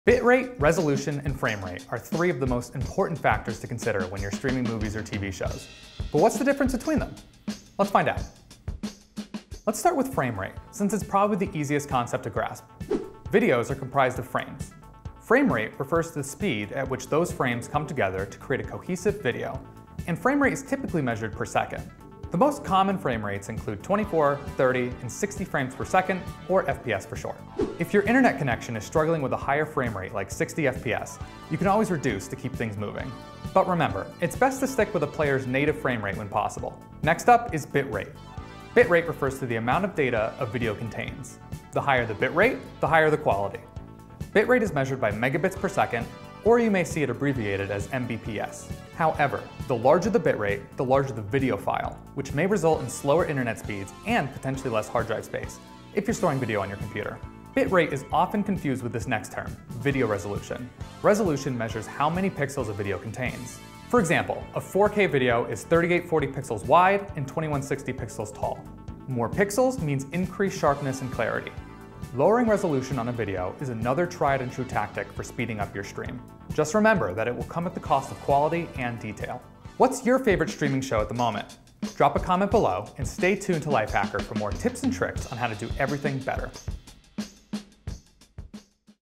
Bitrate vs Frame Rate vs Resolution | Why It Matters for Streaming
Bitrate, frame rate, and video resolution are all common tech terms. But what do they really mean? And how will they affect your video streaming?

In this video, we'll define each term and help you to understand why they matter.